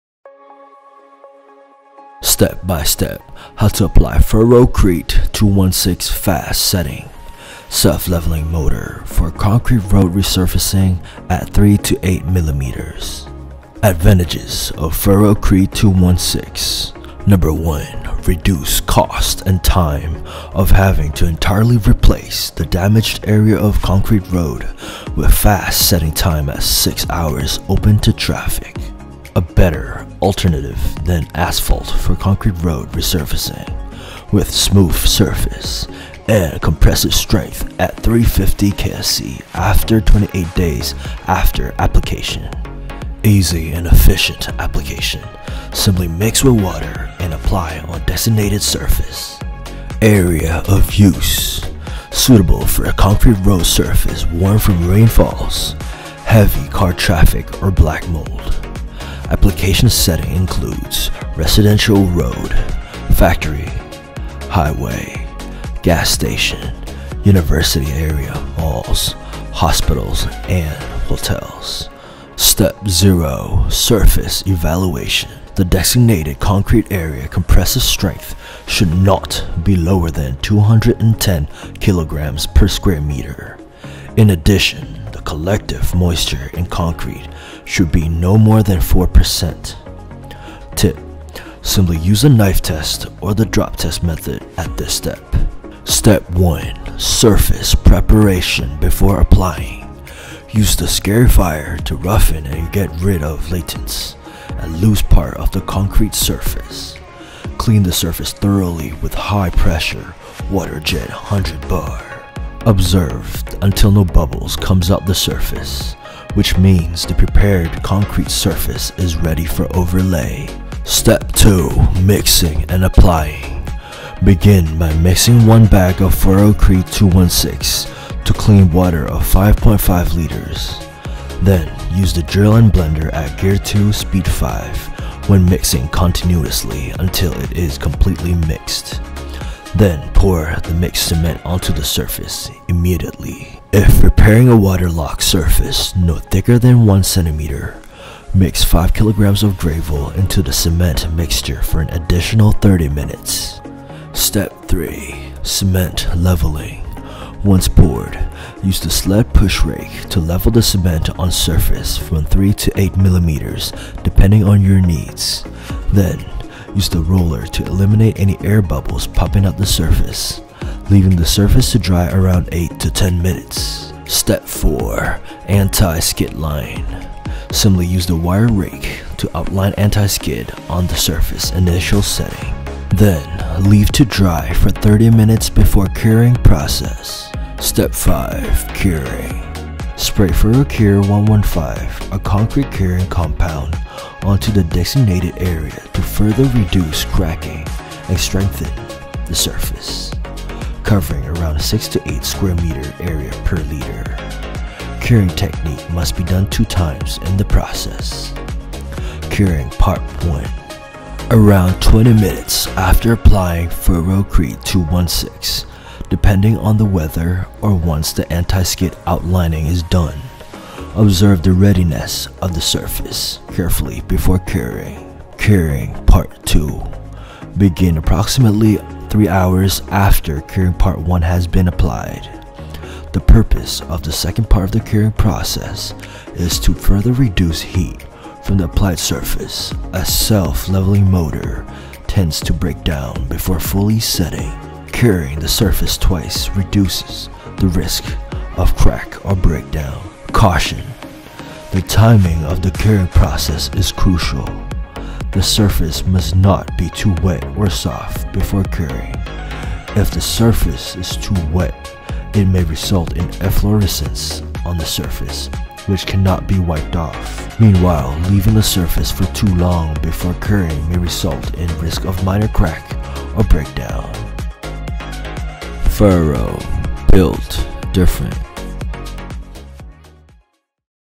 (ENG) Step by Step how to apply "Ferrocrete 216 Fast Setting" Self Leveling Mortar
FERROCRETE 216 is a fast-setting, self-leveling mortar formulated for temporary concrete road resurface. Equipped with high compressive strength up to 350 ksc, Ferrocrete 216 is made of special cement and polymer suitable for outdoor repairs at 3-8 mm. Ferrocrete 216 is a high performance concrete surface repair product that save time and labour cost with fast setting time at 6 hours (open to traffic)

Timestamps:
0:00 Intro
0:40 Step 0. Surface Evaluation
0:58 Step 1. Surface Preparation
1:14 Step 2. Mixing and Apply
1:32 Step 3. Cement Leveling
1:51 Step 4. Anti Skid
2:01 Step 5. Curing
2:35 Caution